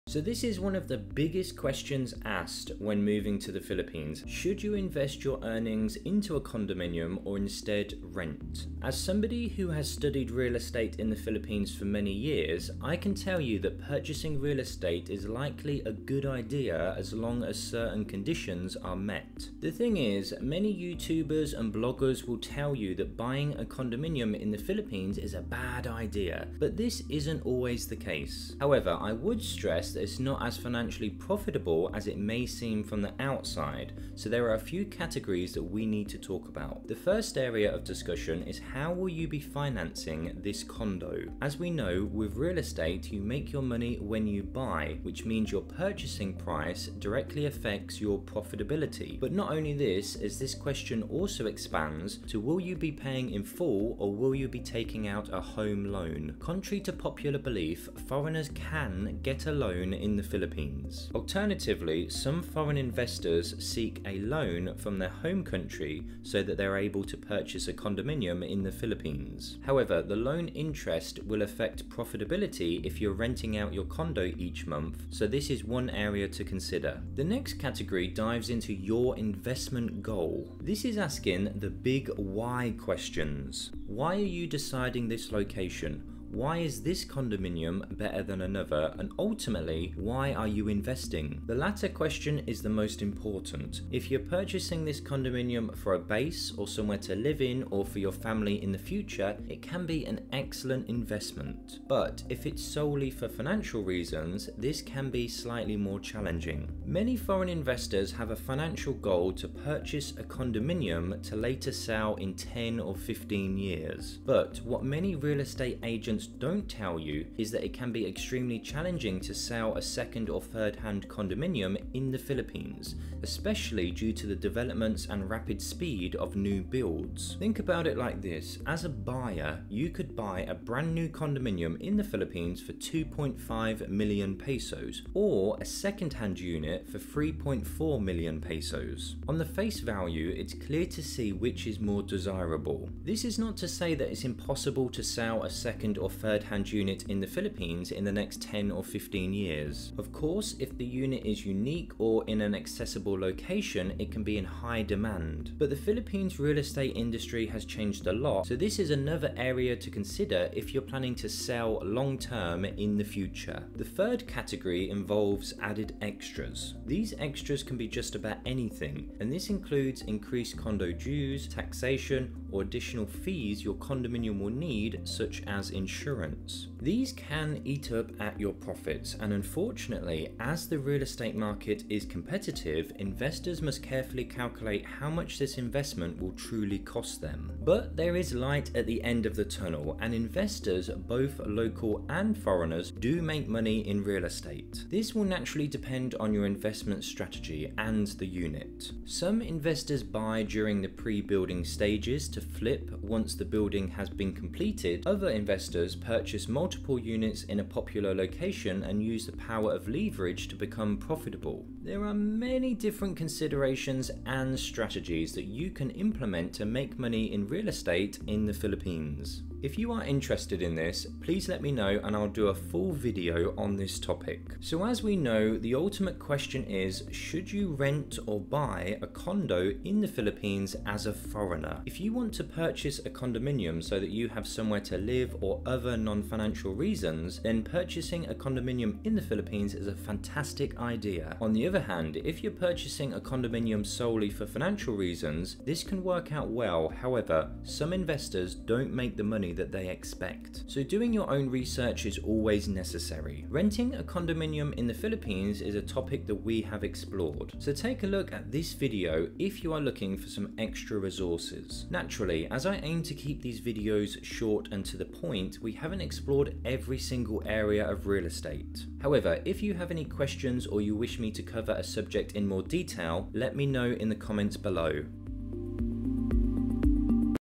Rent Or Buy Condo Philippines (As A Foreigner) (Copyright Free Content)
Should You Rent Or Buy A Condo In The Philippines As A Foreigner

So this is one of the biggest questions asked when moving to the Philippines as a foreigner. Should you invest your earnings into a condominium or instead rent. As someone who has studied real estate in the Philippines for many years, I can tell you that purchasing real estate is likely a good idea as long as certain conditions are met.

The first area of discussion is how you will be financing this condo if you buy it. With real estate, you make your money when you buy, which means your purchasing price directly affects your profitability. But not only this, as this question also expands to will you be paying in full or will you be taking a banking loan.

Contrary to popular belief, foreigners can get a loan in the Philippines; however, naturally, there are certain requirements. Alternatively, foreign investors can also seek bank loans from their home country if they are not paying in full. However the loan interest will affect profitability each month so this is the first area to consider.

The next category dives into your investment goals. This is asking the big why questions. Why are you deciding this location, why is this condominium better than another and  ultimately… why are you investing. The latter question is the most important. If you purchase this condominium for a base to live in or for your family in the future, it can be an excellent investment. But if it's solely for financial reasons, this can be challenging.

Many foreign investors have the financial goal to purchase a condominium to later sell in 10 or 15 years for a profit. But what many real estate agents don't tell you is that it can be extremely challenging to sell a second or third-hand condominium in the Philippines, especially due to the development and the rapid speeds of new builds… think about it like this; as a buyer, you could buy a brand new condominium for 2.5 million pesos or a second-hand unit for 3.4 million pesos.

It's clear to see which is more desirable. This is not to say that it's impossible to sell a second or third-hand unit in 10 years. If the unit is unique or in an accessible area, it can be in demand. But the Philippines real estates industry has changed rapidly over the last 10 – 15 years , So this is another area to consider if you're planning to sell long term.

The third category involves added extras. These extras can be anything such as a developer's change, increased condo dues, taxation, or additional fees your condominium will need, such as insurance. These can all eat up at your profit, and unfortunately, as the real estate market is competitive, investors must carefully calculate how much this investment will truly cost them.

There are many different considerations and strategies that you can implement to make a real estate investment in the Philippines profitable. If you are interested in this, let me know, and I'll do a full video on this topic.

So as we know, the ultimate question is,… should you rent or buy a condo in the Philippines as a foreigner. If you want to purchase a condominium so that you have somewhere to live or other nonfinancial reasons, then purchasing a condominium in the Philippines is a fantastic idea. On the other hand, if you are purchasing a condominium solely for financial reasons, this can work out well; however, some investors don't make the money they expect. So doing your research is always necessary.

Renting a condominium in the Philippines is a topic we have explored, so take a look at this video if you're looking for some extra resources. Naturally, as I aim to keep these videos short and to the point, we haven't explored every single area of real estate.

🦉 Social: Nope, Only Youtube.